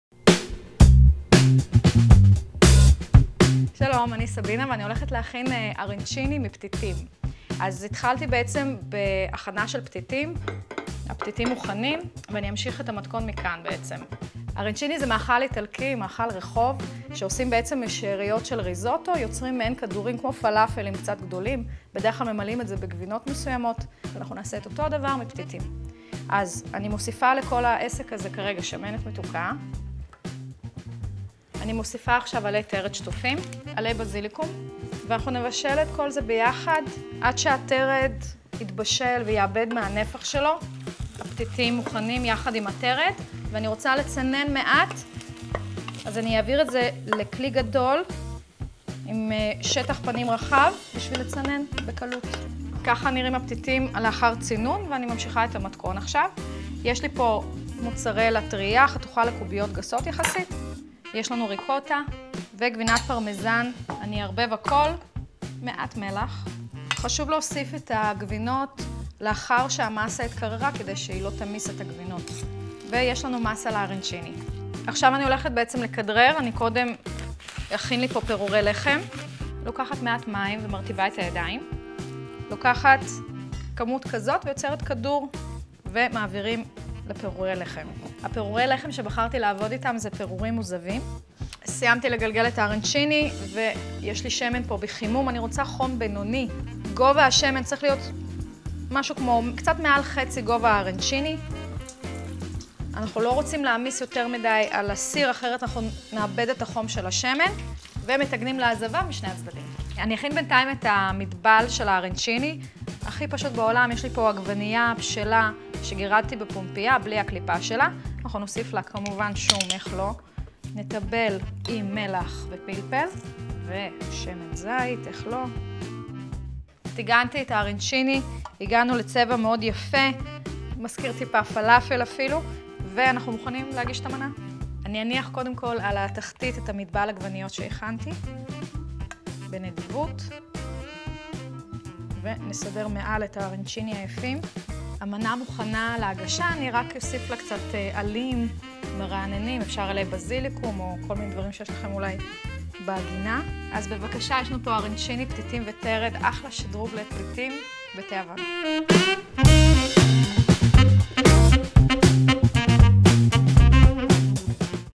מתכוני סוגת: ארנצ'יני פתיתים עם תרד וגבינות
המאכל האיטלקי האהוב בגרסה ישראלית מפתיעה, עם פתיתים מבית סוגת. למתכון